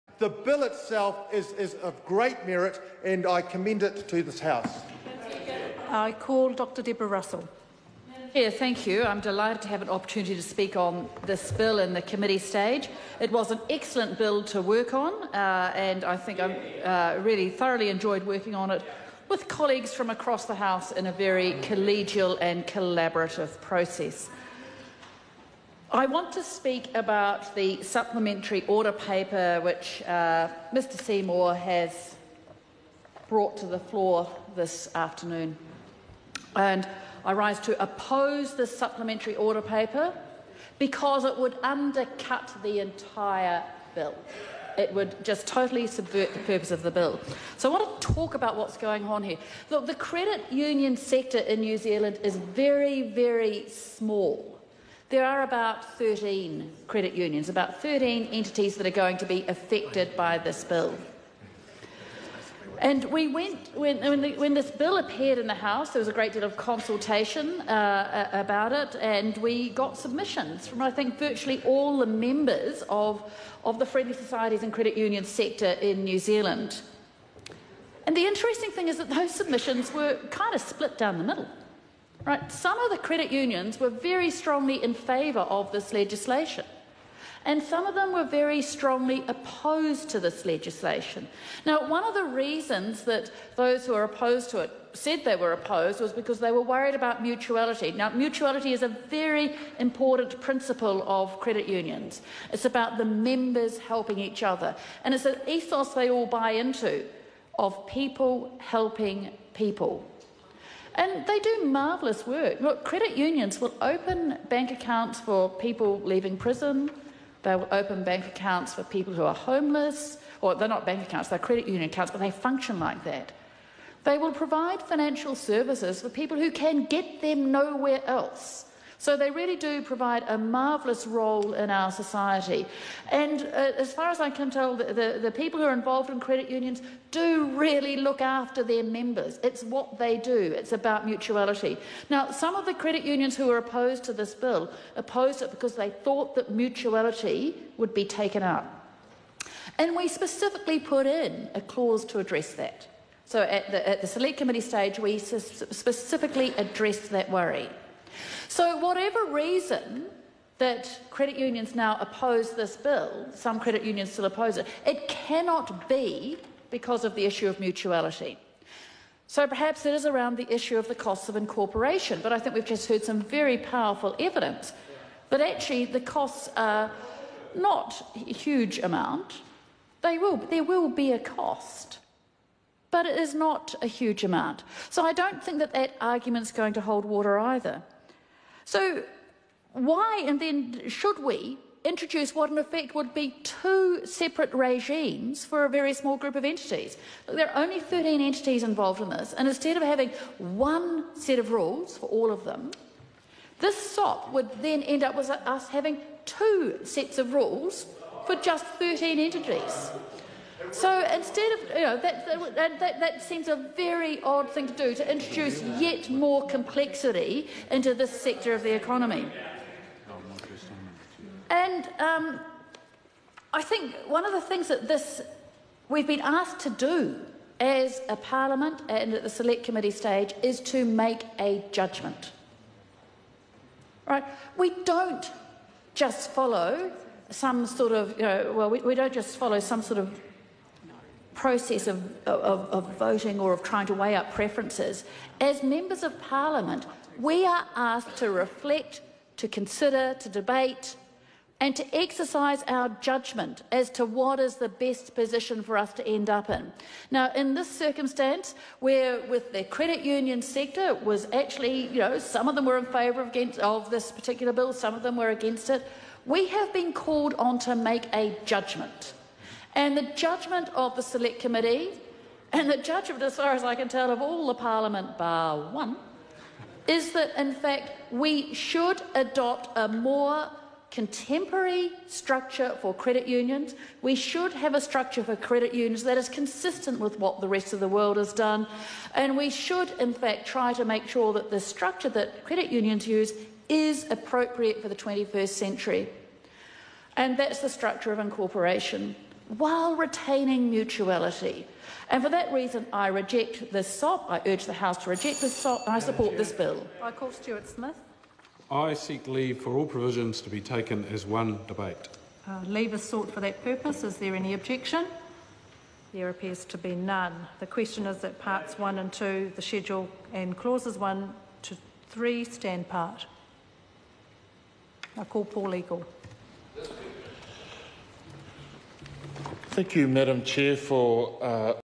Friendly Societies and Credit Unions (Regulatory Improvements) Amendment Bill - Committee Stage - ..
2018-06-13 - Friendly Societies and Credit Unions (Regulatory Improvements) Amendment Bill - Committee Stage - Video 5

Deborah Russell